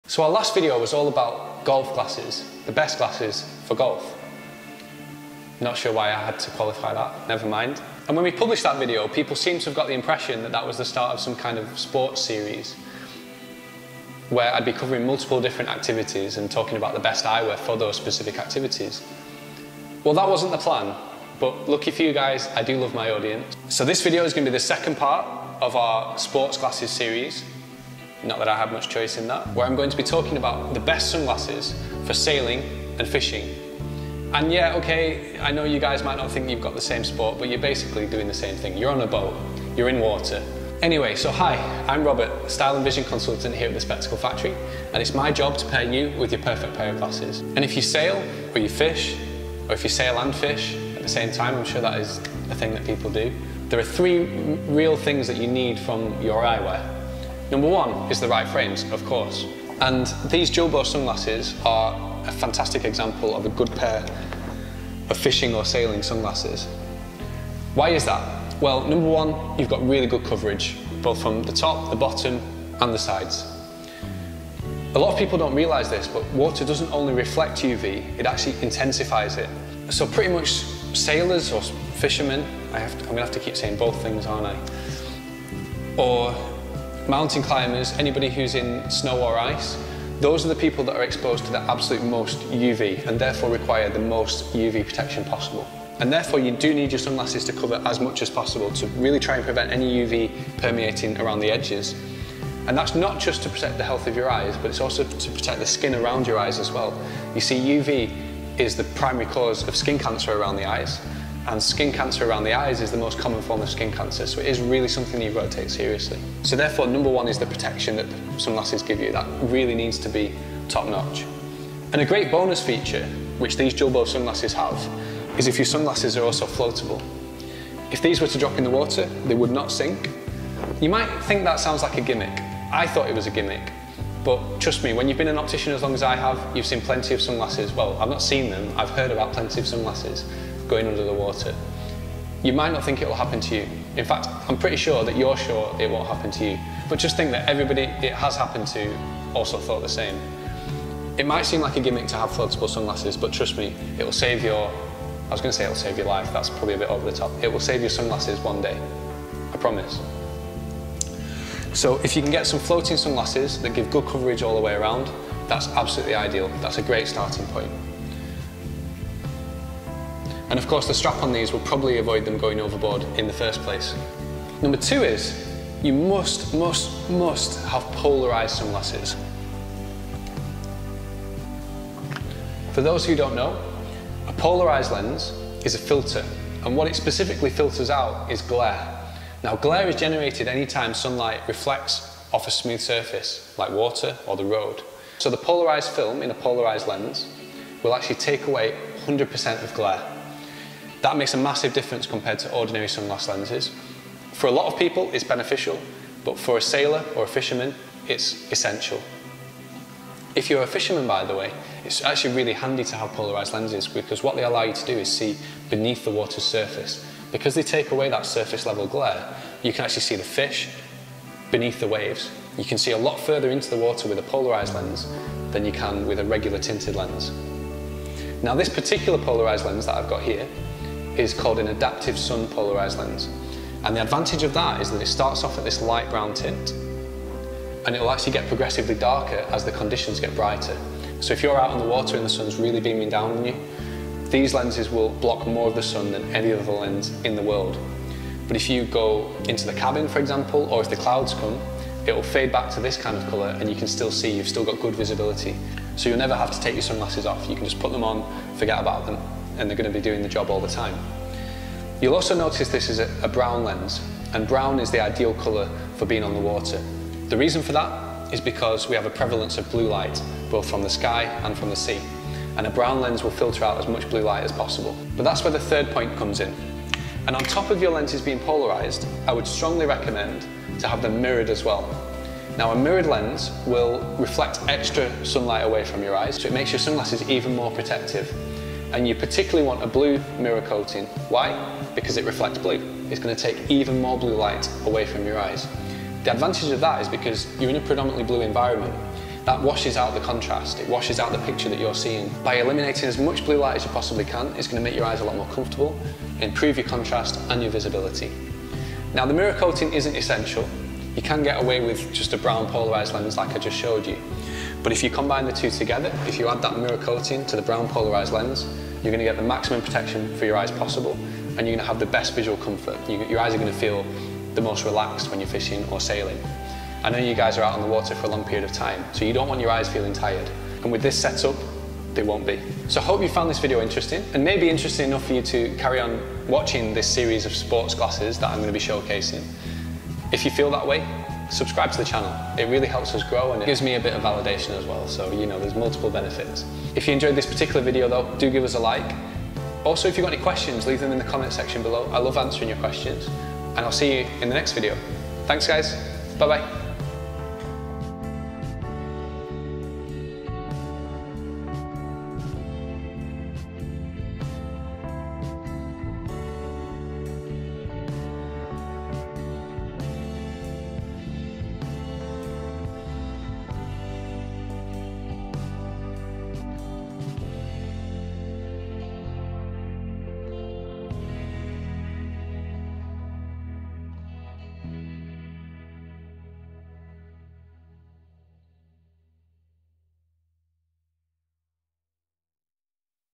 What Sunglasses are Best for Sailing or Fishing
Good quality sunglasses are essential on the water. In this video we cover the 3 things that you need to get the best sunglasses for sailing or fishing.